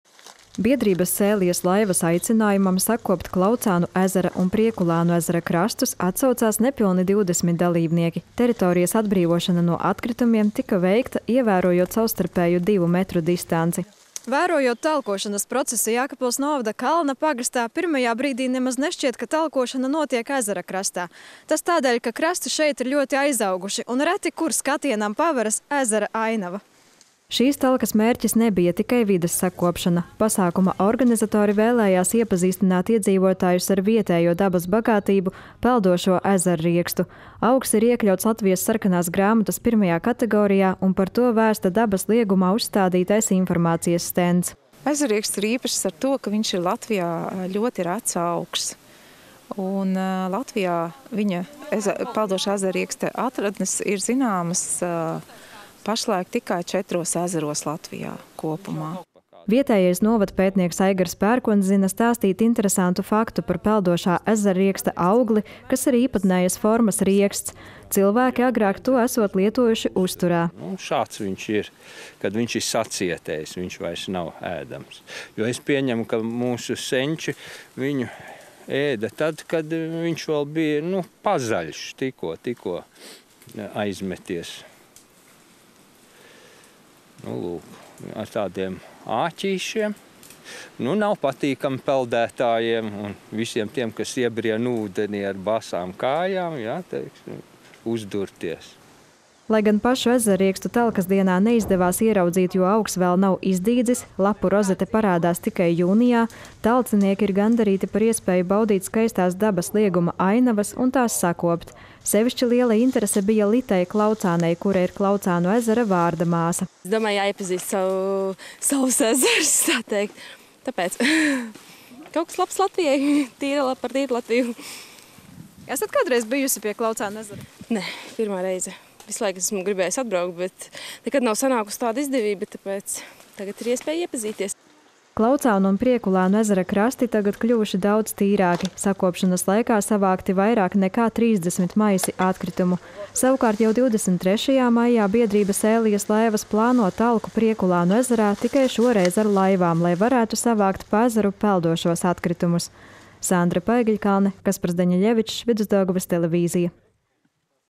Izglīto par aizsargājamu un retu augu Latvijā – peldošo ezerriekstu
Biedrības “Sēlijas laivas” aicinājumam sakopt Klaucānu ezera un Priekulānu ezera krastus atsaucās nepilni 20 dalībnieki. Teritorijas atbrīvošana no atkritumiem tika veikta, ievērojot savstarpēju divu metru distanci. Šīs talkas mērķis nebija tikai vides sakopšana. Pasākuma organizatori vēlējās iepazīstināt iedzīvotājus ar vietējo dabas bagātību – peldošo ezerriekstu. Augs ir iekļauts Latvijas Sarkanās grāmatas 1.kategorijā, un par to vēsta dabas liegumā uzstādītais informācijas stends.

“Ezerrieksts ir īpašs ar to, ka tas ir Latvijā ļoti rets augs. Peldošā ezerrieksta atradnes pašlaik ir zināmas tikai četros ezeros Latvijā kopumā,” stāsta Dabas aizsardzības pārvaldes Latgales reģiona vecākā valsts vides inspektore Irēna Skrinda.

Vietējais novadpētnieks Aigars Pērkons zina stāstīt interesantu faktu par peldošā ezerrieksta augli, kas ir īpatnējas formas rieksts. Cilvēki agrāk to esot lietojuši uzturā. Vienu riekstu Aigars Pērkons savulaik atradis pie Priekulānu ezera: “Šāds tas izskatās, kad ir sacietējis. Tad vairs nav ēdams. Pieņemu, ka mūsu senči to ēda tad, kad tas vēl bija zaļš, tikko aizmeties. Rieksts ir ar “āķīšiem”, tādēļ visiem, kas iebrien ūdenī ar basām kājām, nav patīkami uz tāda uzdurties.”

Lai gan pašu ezerriekstu talkas dienā neizdevās ieraudzīt, jo augs vēl nav izdīdzis – lapu rozete parādās tikai jūnijā, talcinieki ir gandarīti par iespēju baudīt skaistās dabas lieguma ainavas un tās sakopt. Sevišķi liela interese bija Litai Klaucānei no Jumpravas, kura ir Klaucānu ezera vārda māsa: “Piedalos, jo domāju, ka jāiepazīst “savs ezers”. Visu laiku esmu gribējusi atbraukt, bet nekad nav sanākusi tāda izdevība, tāpēc tagad ir iespēja iepazīt.”

Klaucānu un Priekulānu ezera krasti tagad kļuvuši daudz tīrāki – sakopšanas laikā savākti vairāk nekā 30 maisi atkritumu. Šī talka bija pirmā no piecām, kas paredzētas biedrības “Sēlijas laivas” īstenotajā projektā “Domā globāli, rīkojies lokāli!”. Turpmākās talkas paredzētas Viesītes pilsētas Mazajā ezeriņā, Elkšņu pagasta Dumbļa purvā, Viesītes pagasta Sūpes purvā, kā arī Dienvidsusējas upē Aknīstes, Elkšņu, Rites un Neretas pagasta robežās. Katrā aktivitātē līdztekus talkošanai paredzētas arī vides lekcijas par Sēlijas dabas vērtībām.

Savukārt jau 23.maijā biedrība “Sēlijas laivas” plāno talku Priekulānu ezerā, tikai šoreiz ar laivām, lai varētu savākt pa ezeru peldošos atkritumus.

Sandra Paegļkalne